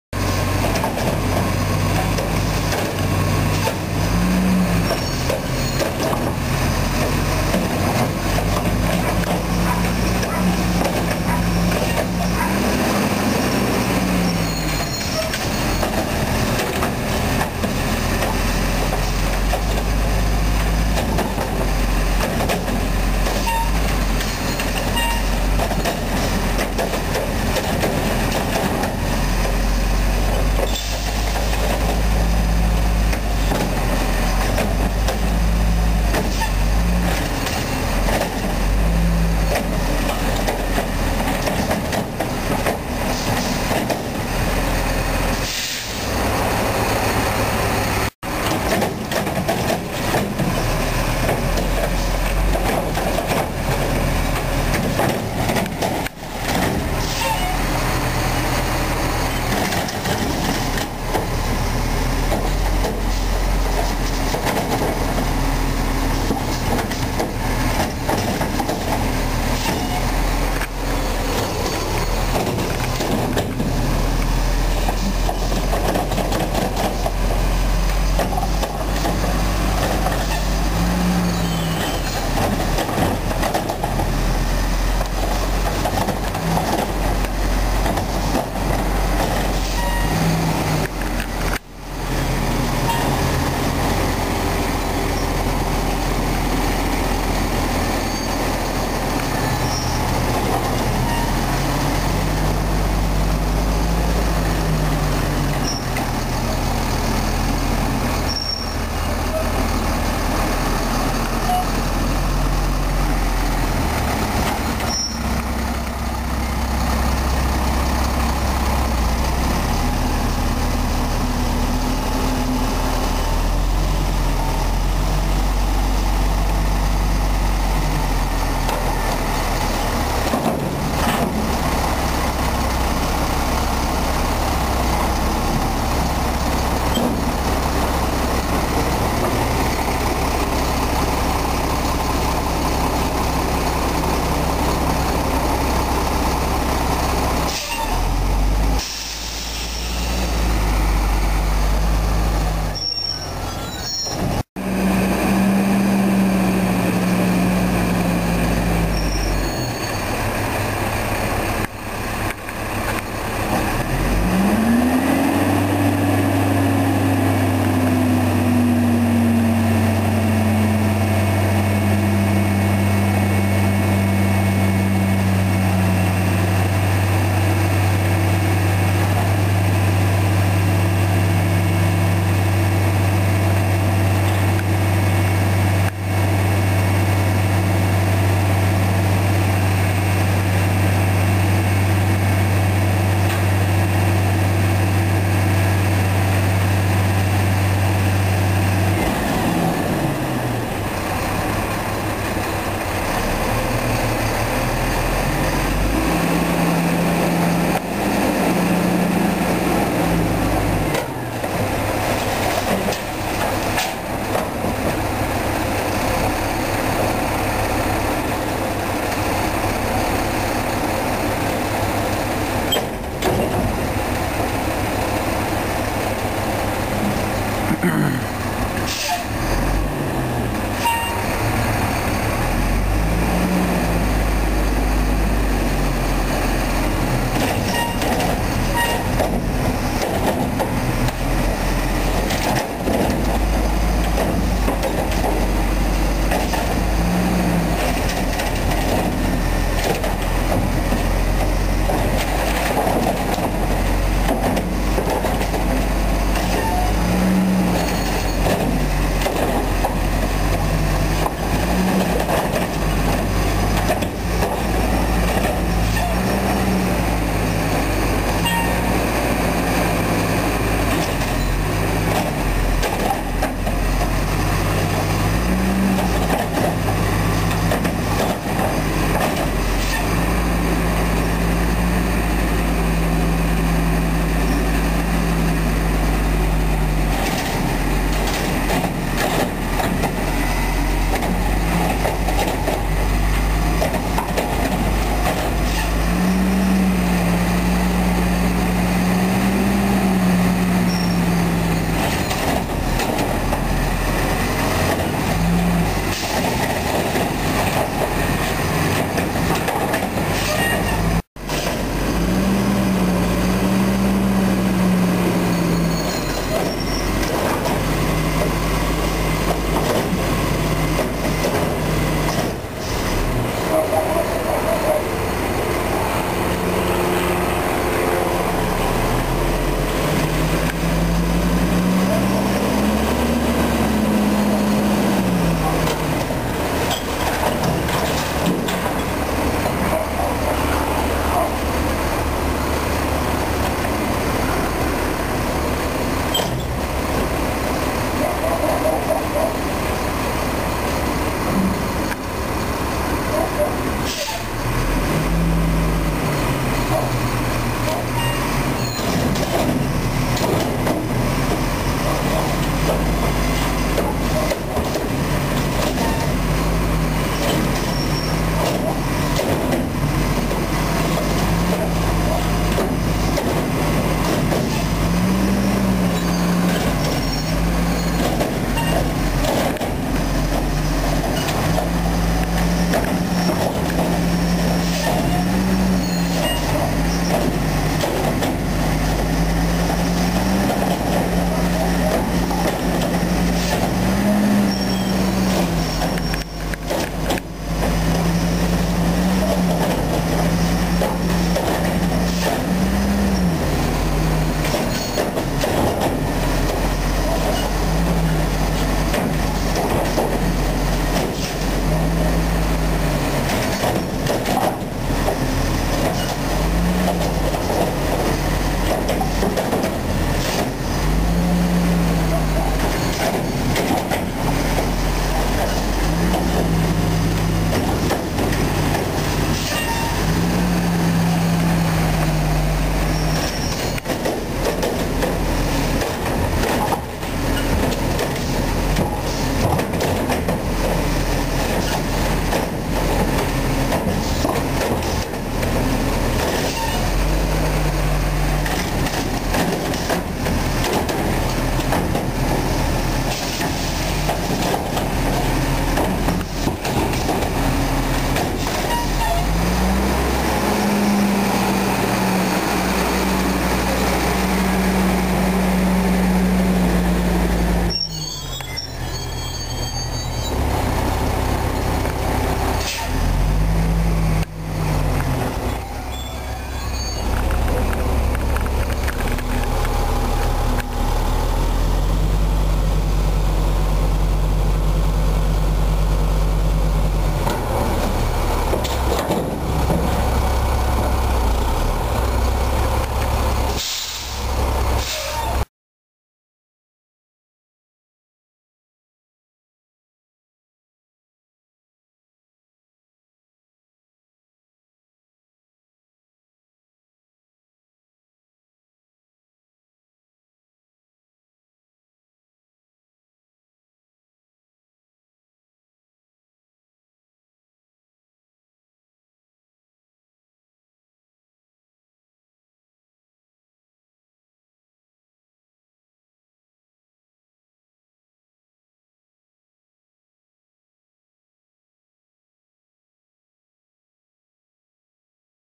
A walk in Signal Hill
So I was dropping off some motorcycles at Long Beach Motorsports and I went on a walk to check out what was going on nowadays out there in Signal Hill. The Bowles is no longer collecting on a regular basis, and now truck 583, a Volvo / CWS Industries body with a Curotto Can is out there now.